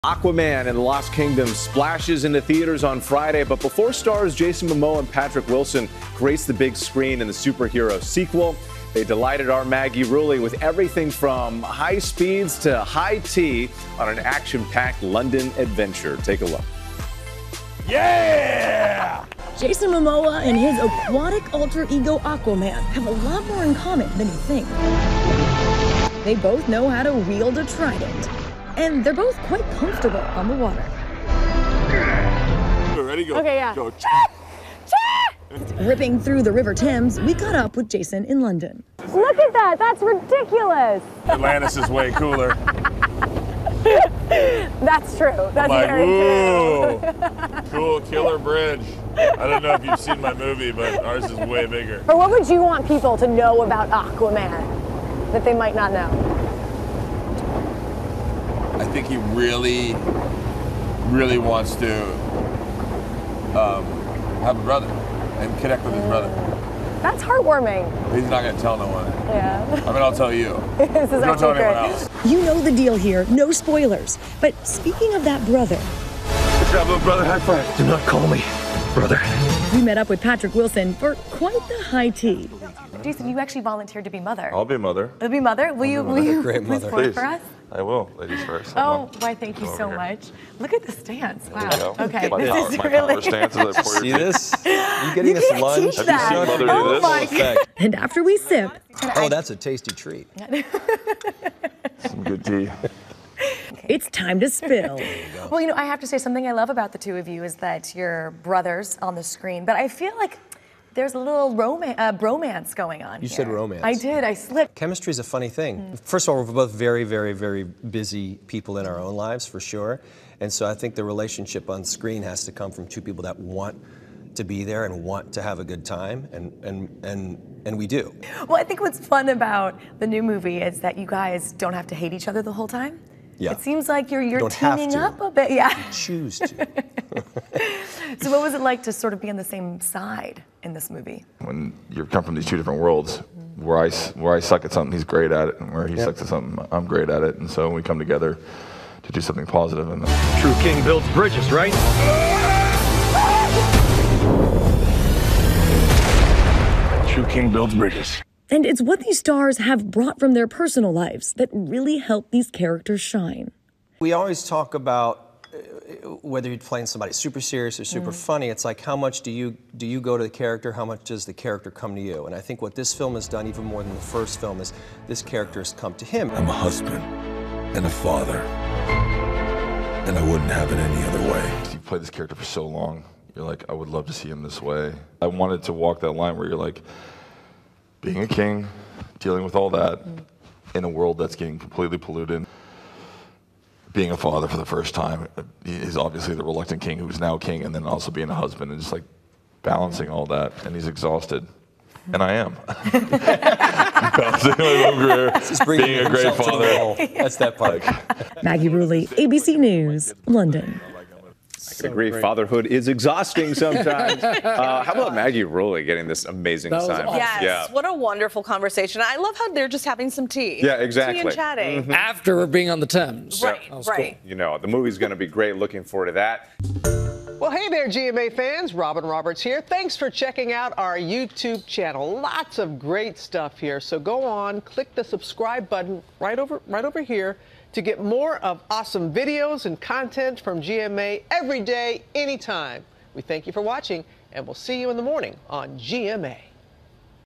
Stars talk 'Aquaman and the Lost Kingdom'
"Aquaman and the Lost Kingdom" splashes into theaters Friday, but before stars Jason Momoa and Patrick Wilson grace the big screen, they delighted our Maggie Rulli on a London adventure.